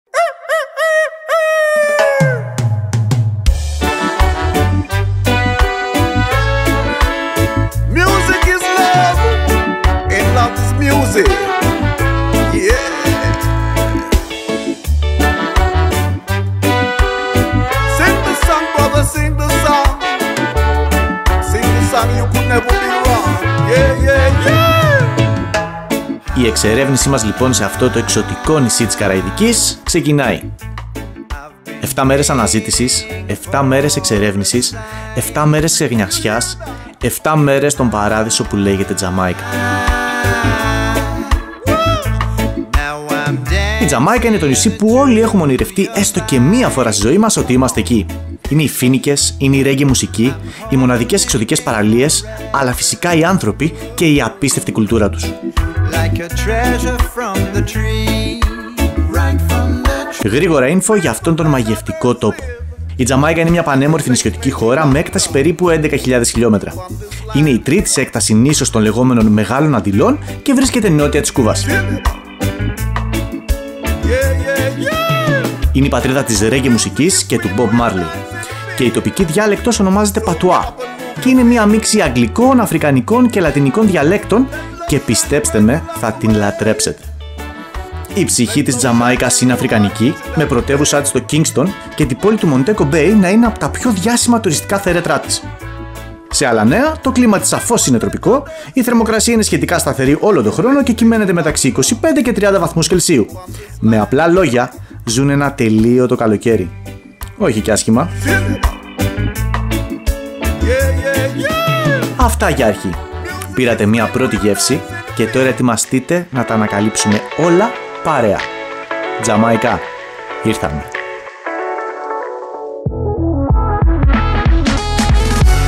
ΚΑΛΑ ΘΑ ΠΑΕΙ ΑΥΤΟ | Τζαμάικα
🇯🇲  Από την Τζαμάικα της Ευρώπης στην Κρήτη της Καραϊβικής!
Η περιπέτεια ξεκίνησε
Οι εικόνες εξωτικές
Οι άνθρωπο αυθεντικοί
Όλα γύρω σου μοιάζουν με εικόνες από καρτ ποστάλ σε ένα τόπο που είναι πάντα καλοκαίρι...
ΤΖΑΜΑΙΚΑ... Ήρθαμε...